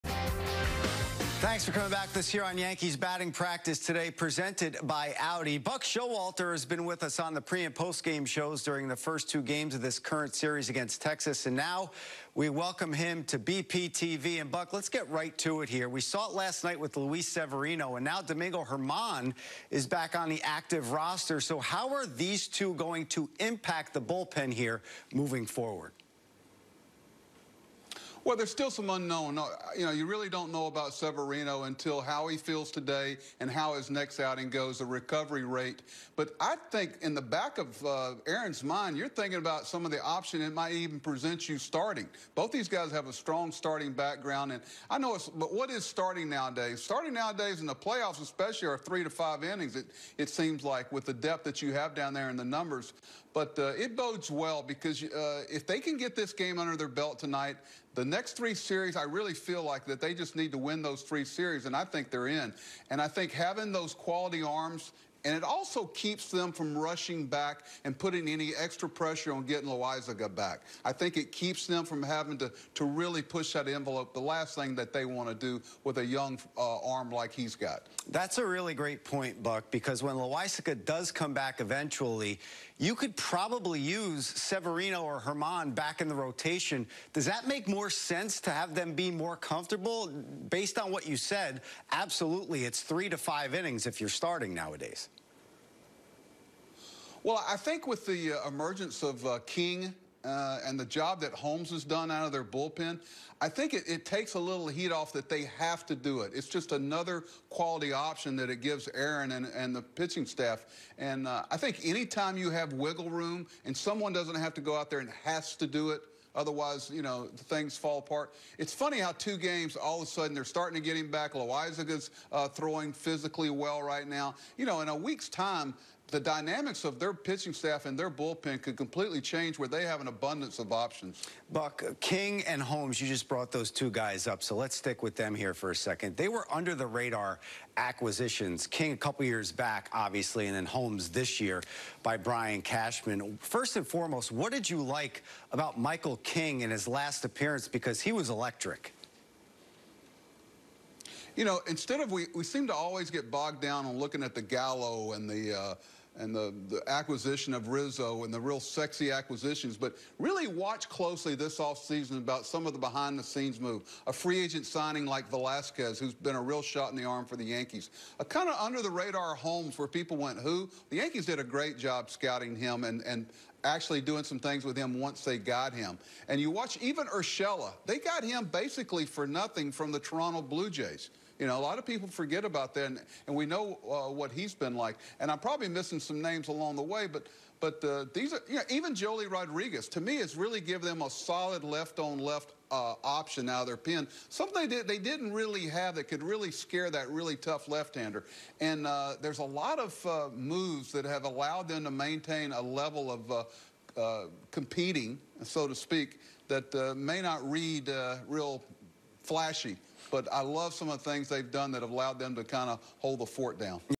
Buck Showalter on the Yankees' bull pen.
With 10 games left in the regular season, Chris Shearn and Buck Showalter break down what the return of Luis Severino and Domingo German mean for the Yankees.